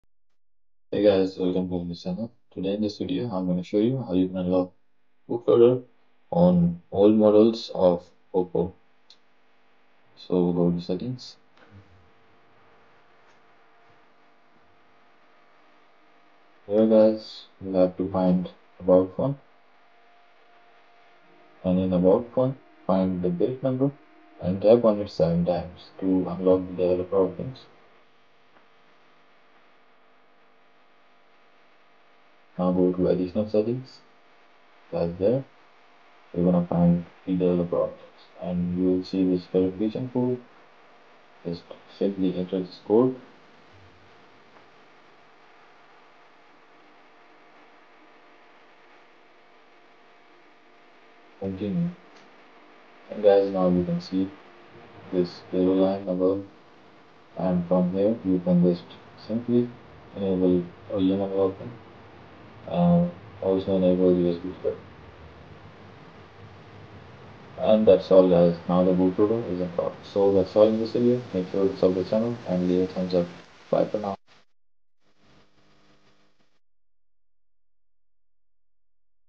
Oppo Bootloader Unlock | Bootloader unlock on any android
in this video i will show you how to unlock bootloader on oppo